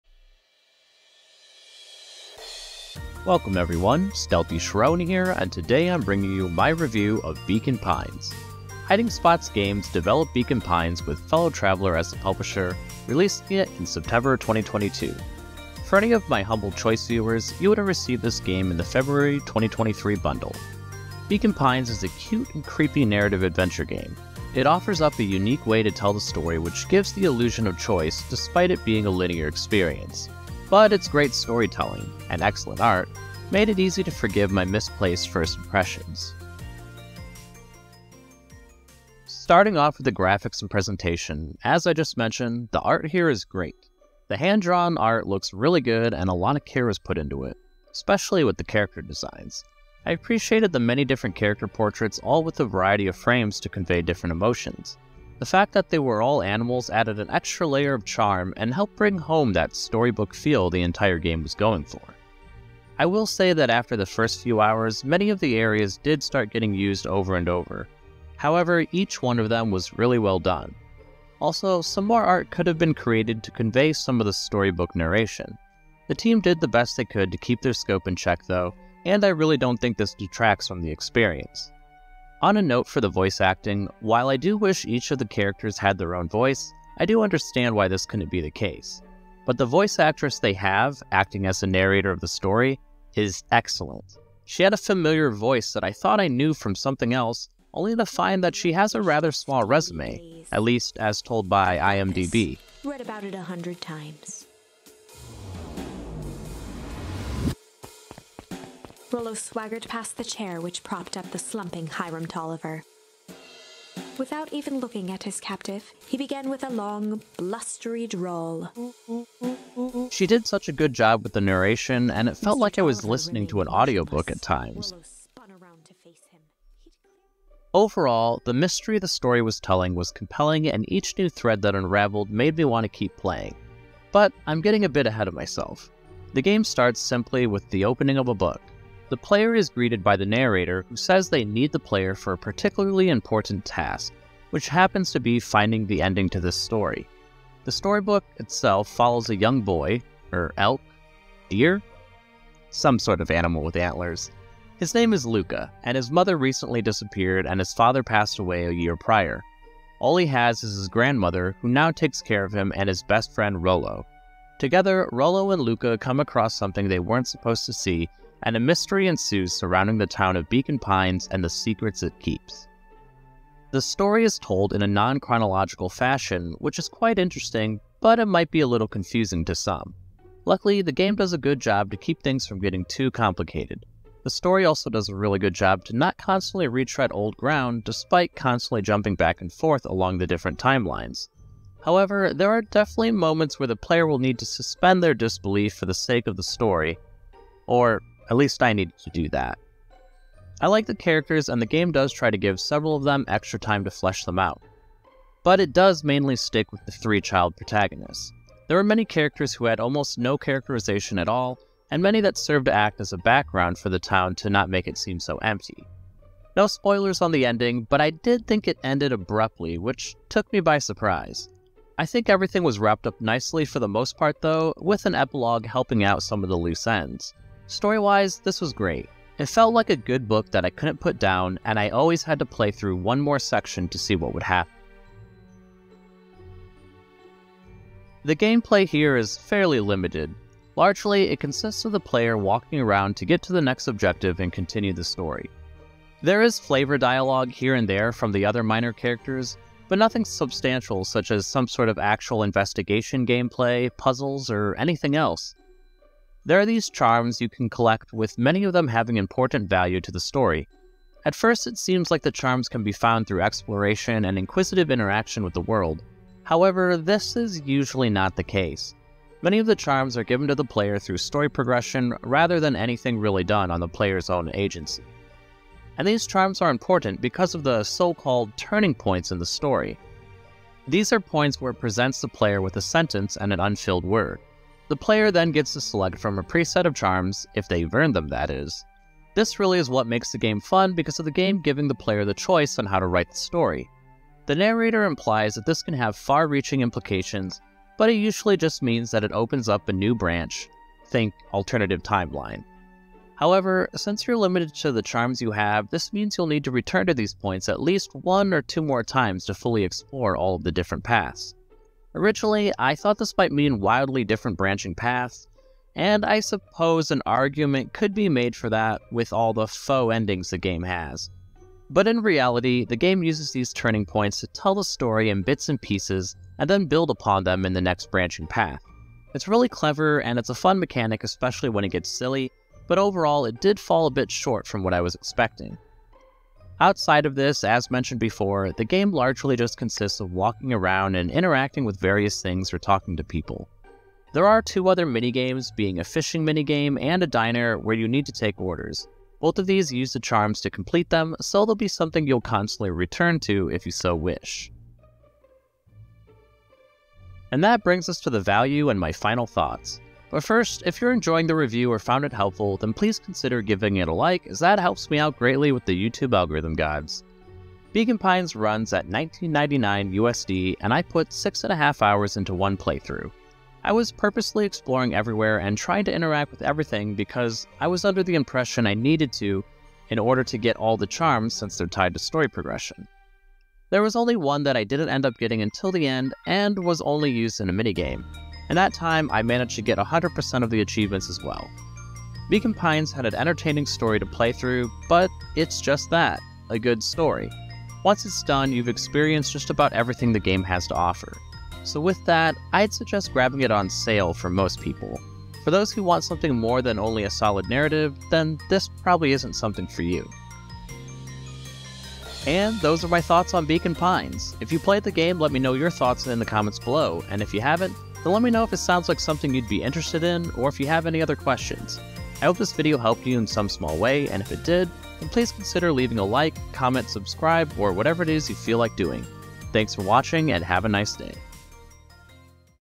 A fine bit of non-linear storytelling - Beacon Pines Review
Hey everyone! Here's my review of Beacon Pines, a charming non-linear narrative adventure game. Let me know your thoughts!

You can tip me with a Thanks under the video!

0:00 Intro
0:39 Graphics/Presentation
2:07 Story/Theme
3:58 Gameplay
5:57 Value and Final Thoughts